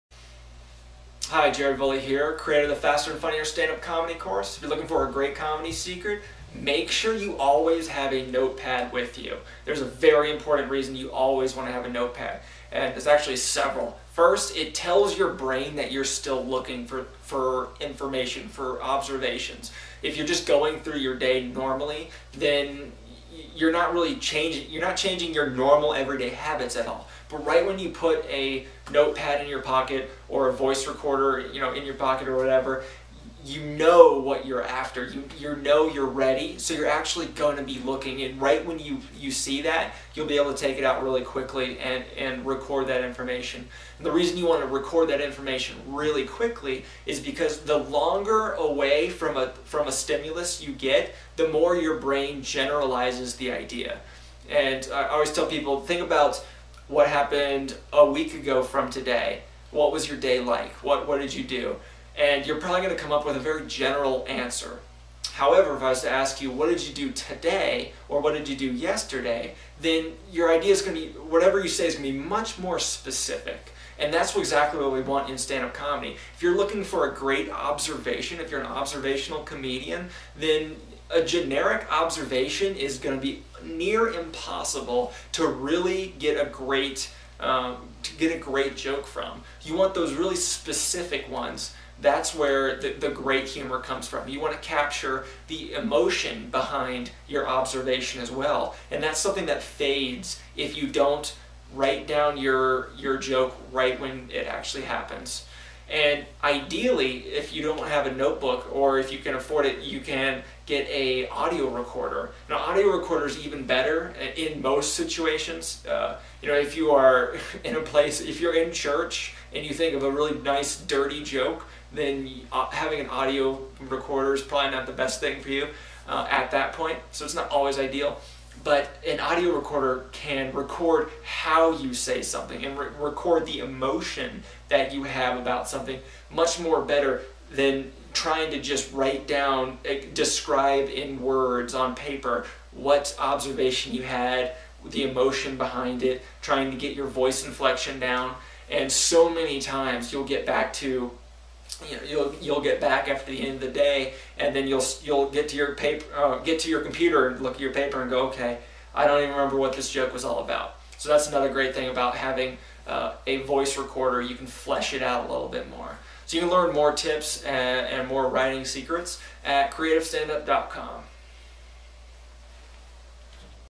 Comedy Writing Secrets- Never Forget a Joke
Discover stand up comedy writing secrets that you can apply immediately to your career Carrying around a notepad to capture your stand up comedy writing ideas is more than just ensuring that you won't forget a joke. It's also key to capturing the nuances behind an observation that take a story from vague and boring to fun and interesting. Applying these comedy writing secrets will give you more material to work with when you make it back to write comedy.